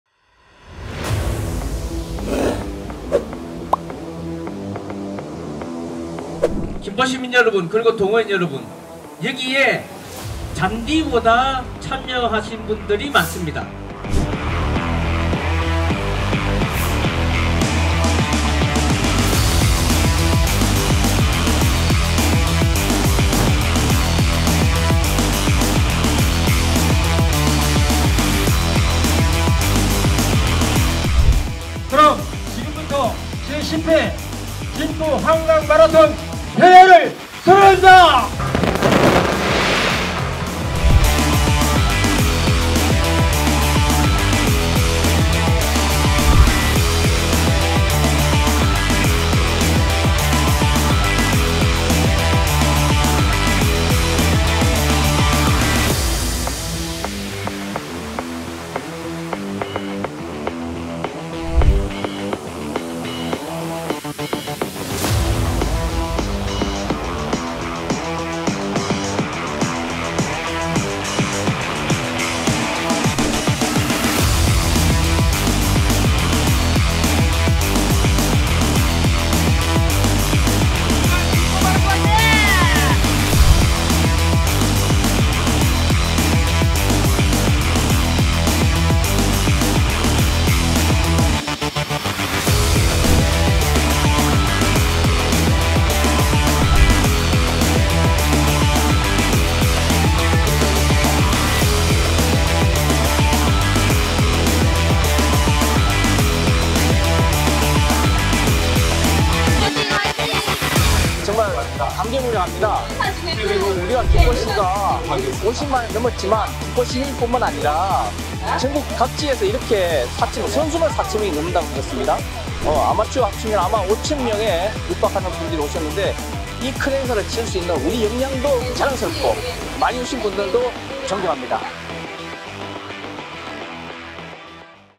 김포시와 함께 상쾌하게 달려요~🏃‍♂️ 제10회 김포한강마라톤대회!
가을 하늘 공활한데~🌤 지난 19일 김포종합운동장에는
'제10회 김포한강마라톤대회' 참가자로 북적였는데요,

3년 만에 올린 이 행사에는 그동안 대회에 참가한 인원 중
가장 많은 3,872명이 모였다고 합니다👍
김포시민뿐만 아니라 타지역 시민들도 모여
따뜻한 햇볕 아래 힘차게 완주에 도전했습니다.

공연 행사까지 더해져 더욱 즐겁고
뜻깊었던 김포한강마라톤대회!🤗

그 뜨거웠던 현장을 영상에서 만나보시죠📺